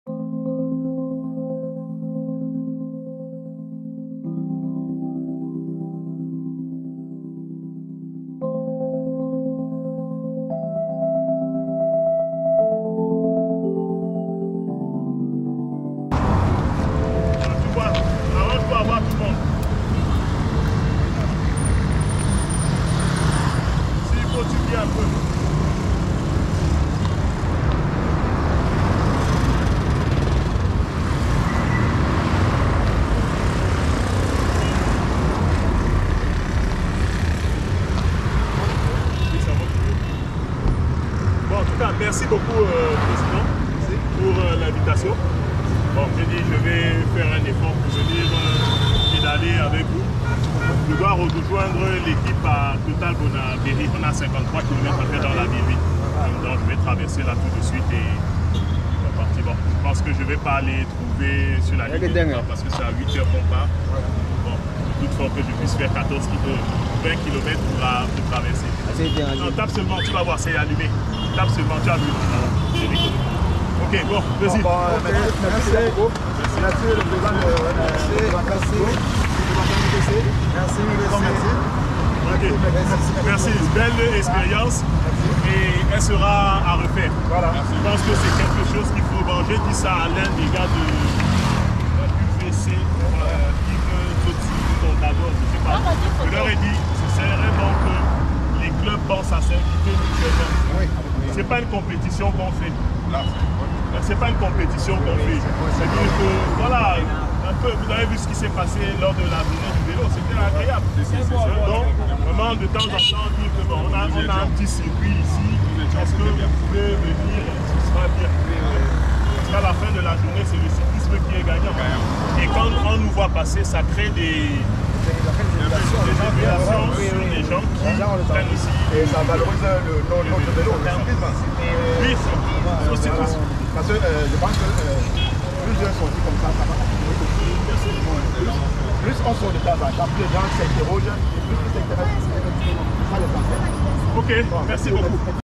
Pause au carrefour Ecole publique de Deïdo
Association Sportive Cyclisme et Loisirs lance sa saison sportive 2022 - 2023
Cyclo Véloclub et Unity Vélo Club ont pris part à cette cérémonie.

vttdreams vous fait revivre cette cérémonie.

Les champions de demain se préparent aujourd'hui